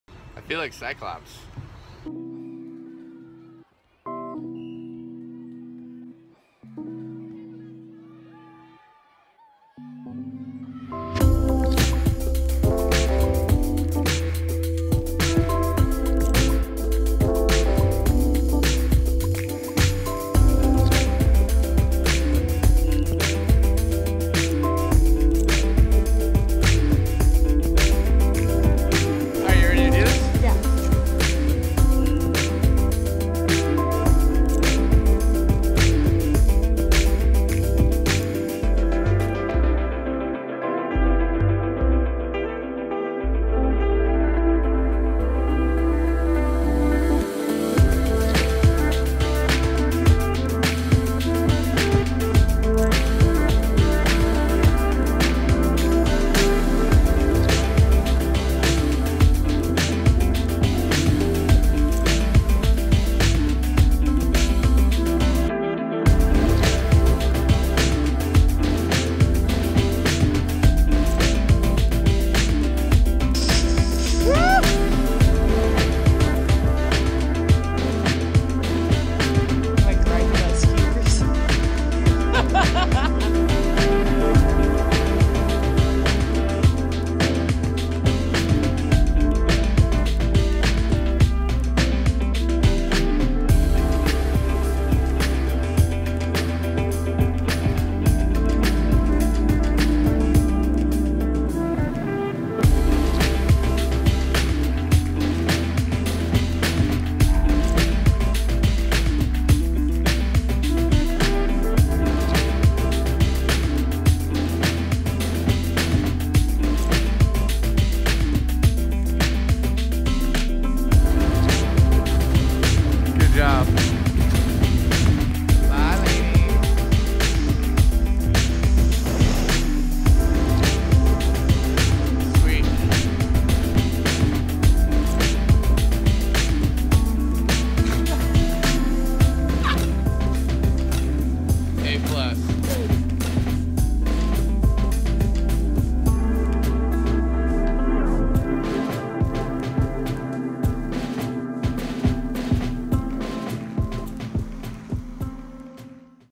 NYC Snow Bus to Hunter Mountain
We took the Snowbus from NYC to Hunter Mountain for a quick snowboarding day trip. And I have to say—it was awesome! Read all about the details in this blog post, and follow along for more sweet NYC day trips at leftyscissor.com